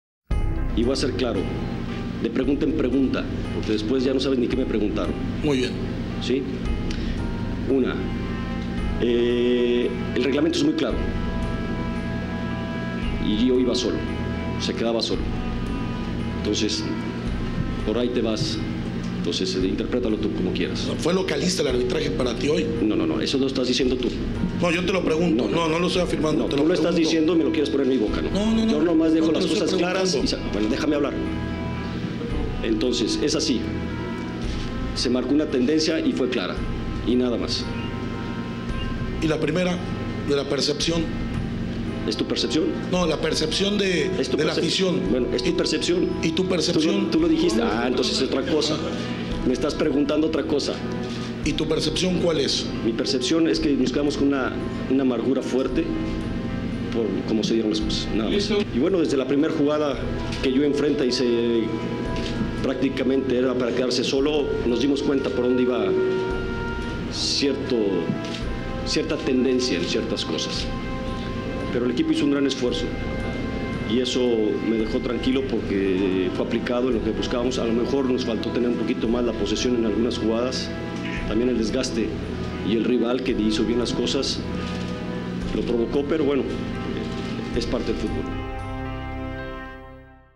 CHEPO EXPLOTA CONTRA UN REPORTERO
José Manuel de la Torre, DT de la Selección Mexicana, se molestó cuando un reportero le cuestionó acerca del arbitraje en el partido en contra de la selección de Hondureña